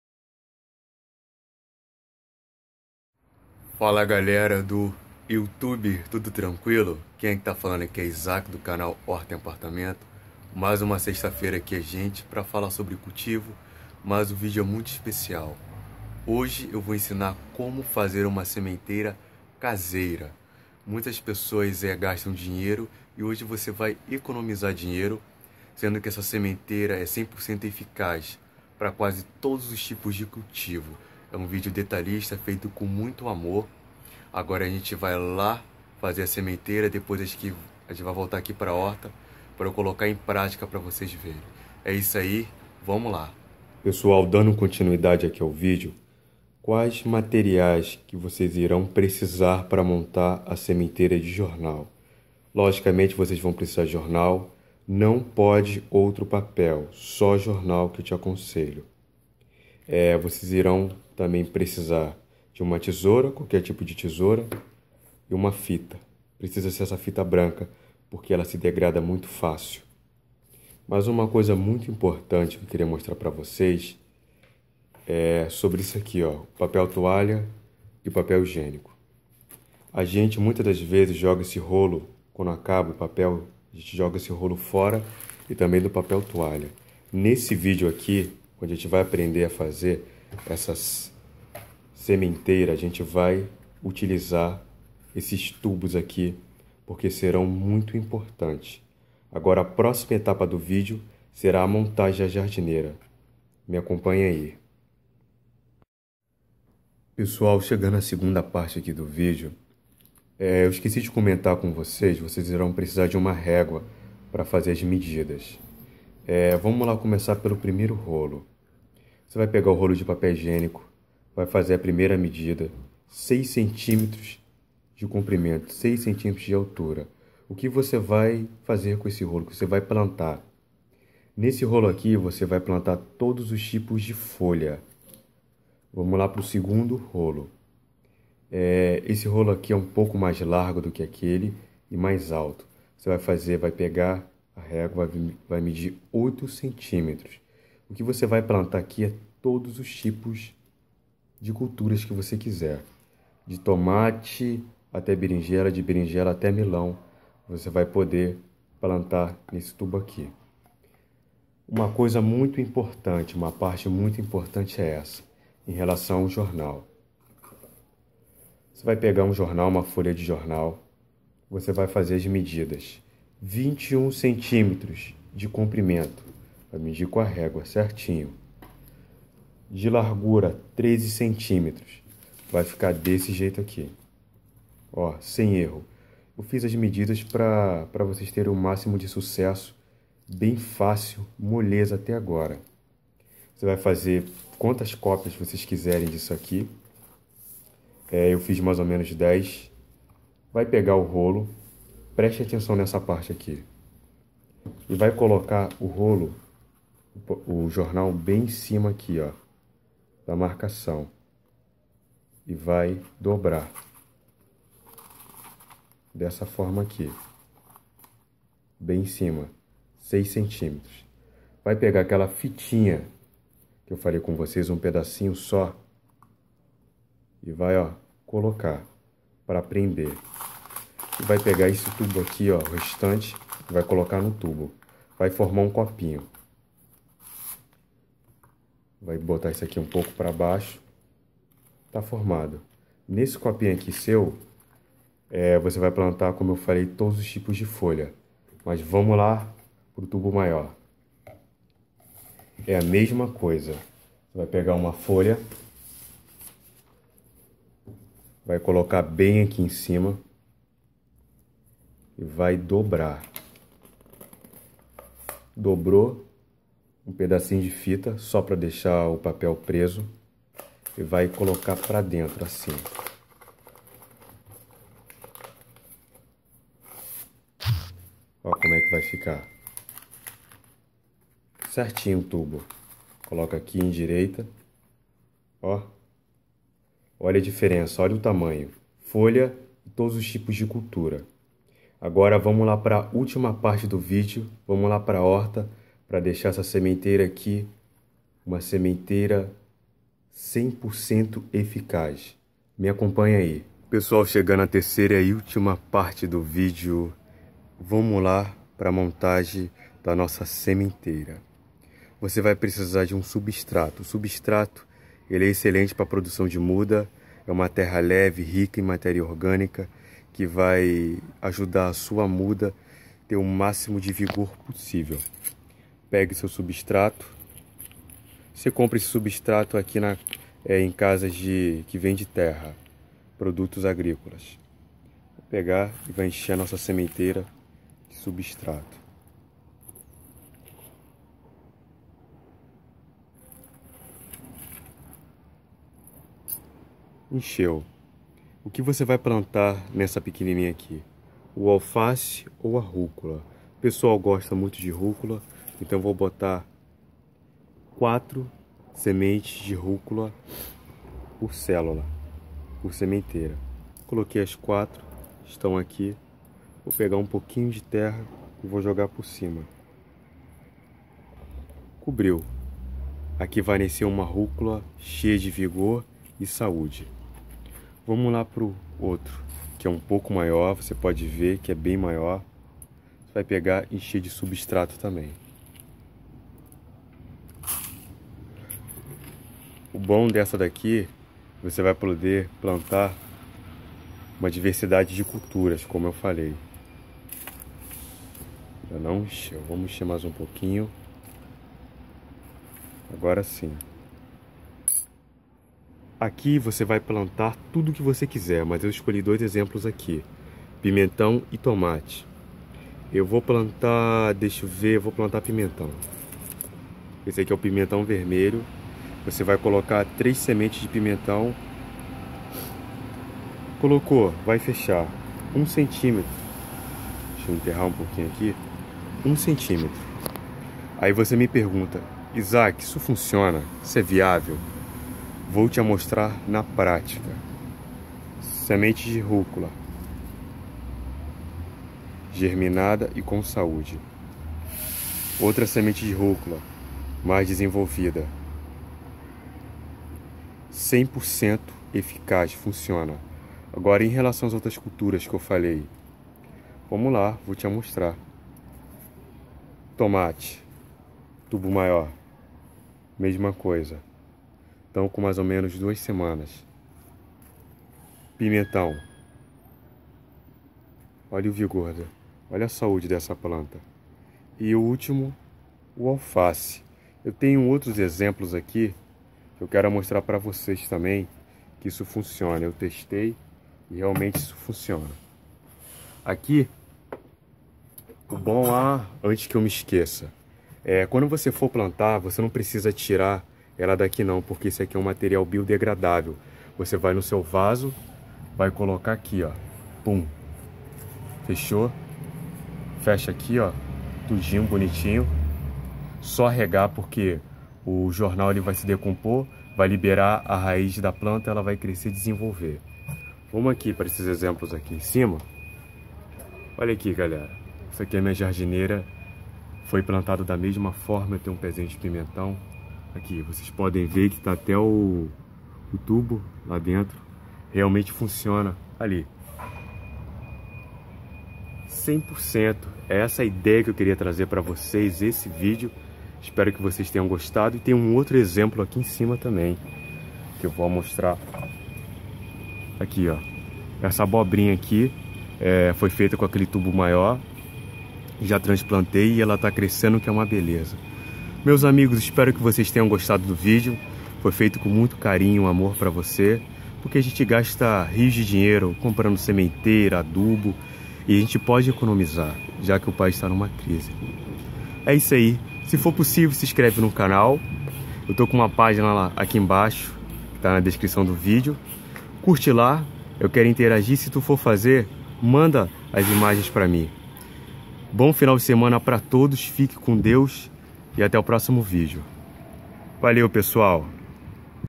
Como Fazer Sementeira de Jornal (inicio,meio,fim)
Pessoa nesse vídeo ensino como fazer uma sementeira 100% sustentável feita unicamente de Jornal . não precisamos mas gastar rios de dinheiro , todo mundo pode ter em casa .